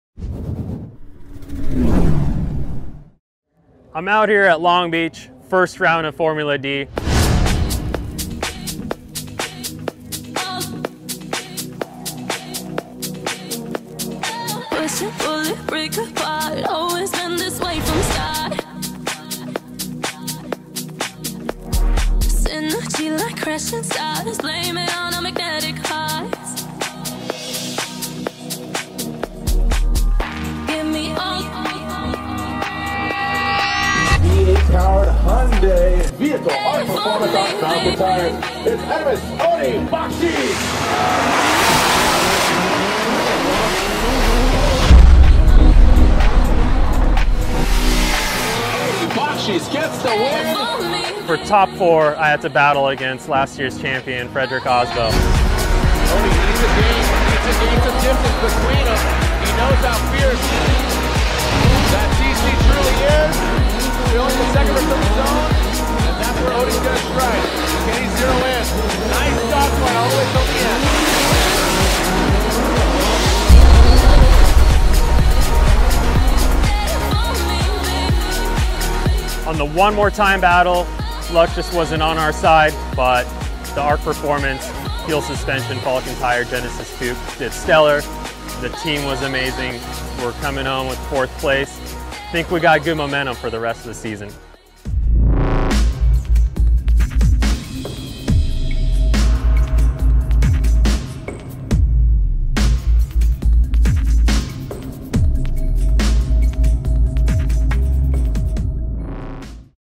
ARK Performance Exhaust at Formula Drift 2016
Formula Drift 2016 Long Beach Recap:
Even though track conditions Odi Bakchis and the team did an amazing job making the right adjustments for the the
ARK Performance Feal Suspension Falken Tire Genesis Coupe to a 4th place finish.